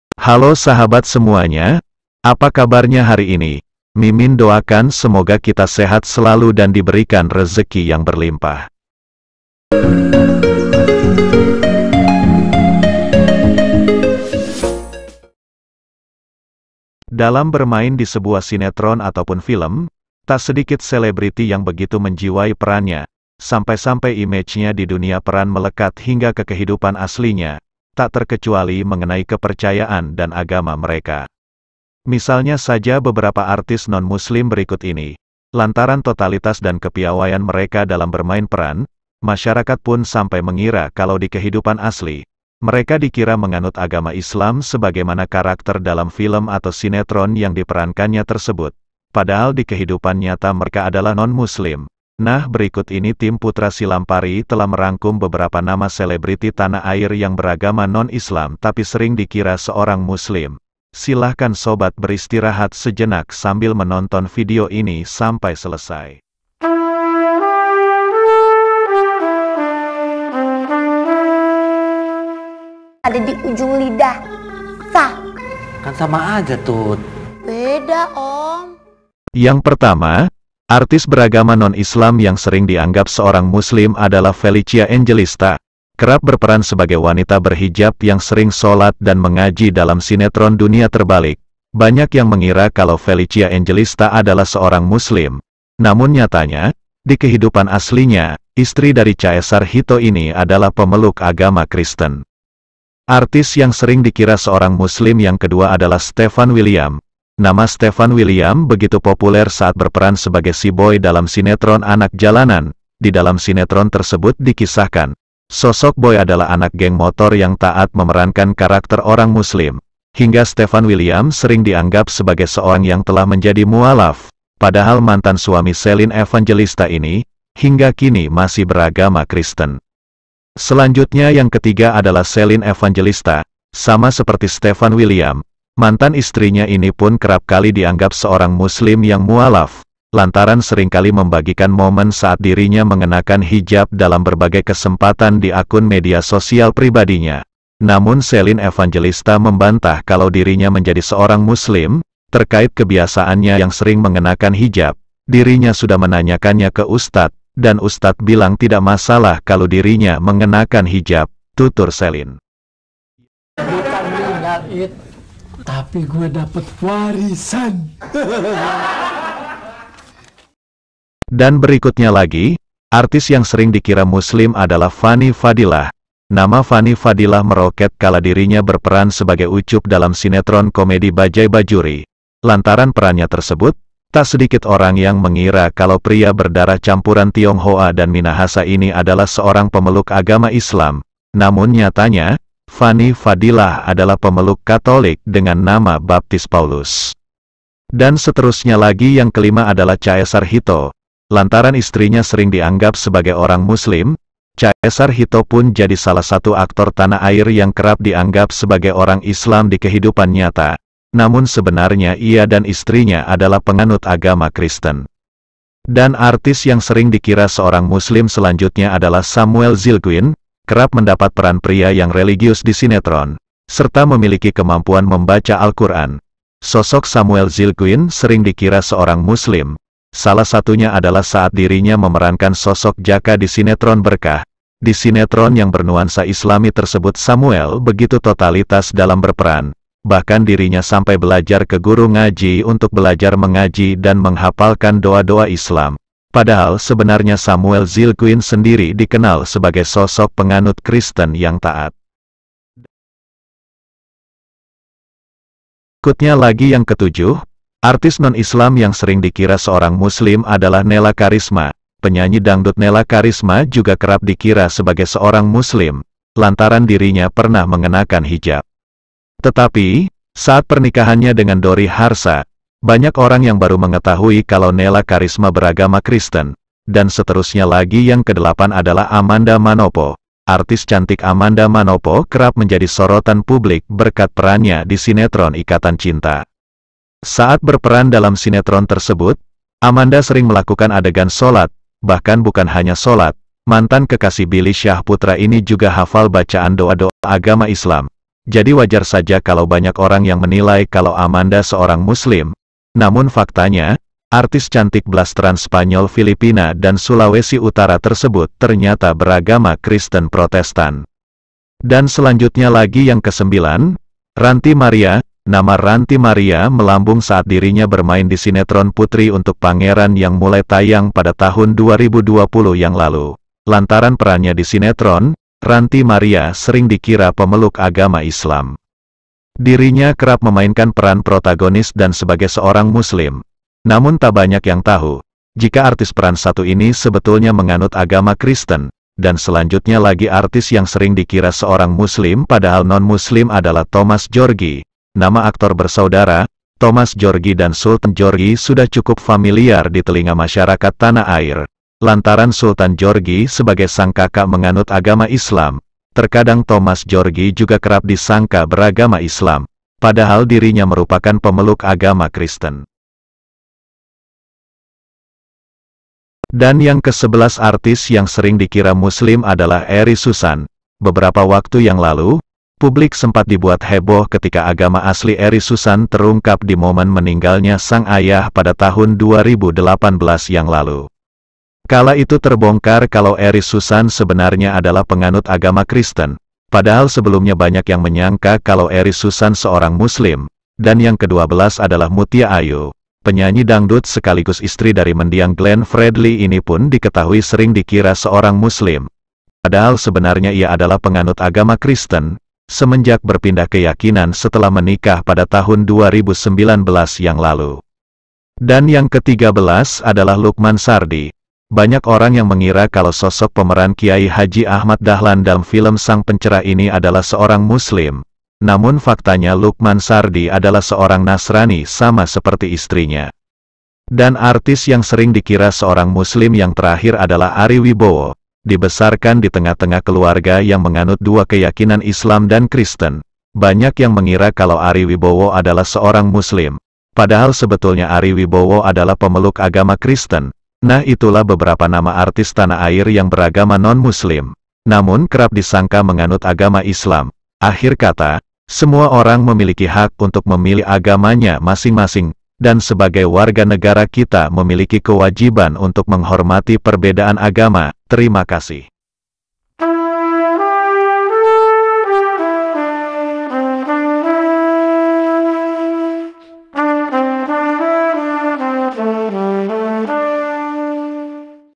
Bukan Islam‼️14 Artis Ini Sering Dikira Seorang Muslim
Berikut ini team putra silampari telah merangkum beberapa nama selebriti tanah air yang bukan Islam tapi sering dikira seorang muslim, silahkan sobat beristirahat sejenak sambil menonton video ini sampai selesai.